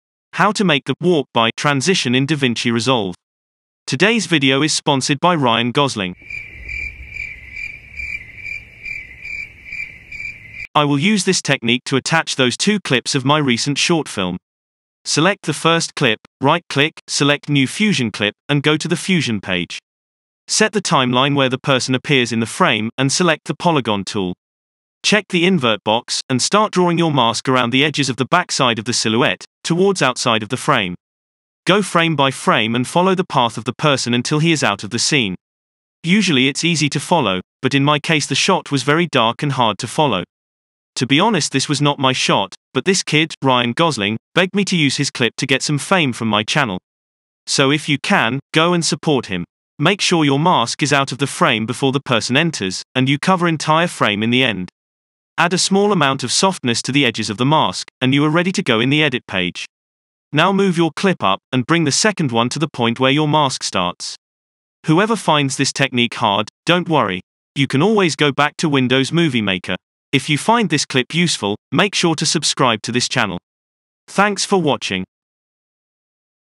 How To Make The Walk By Transition | DaVinci Resolve 17
A simple way to make the walk by transition inside DaVinci resolve 17 in fusion page.

If you are thinking to buy a new camera you can try one of those great cameras :
Sony Alpha 7S III
Blackmagic pocket 6K
sony a6500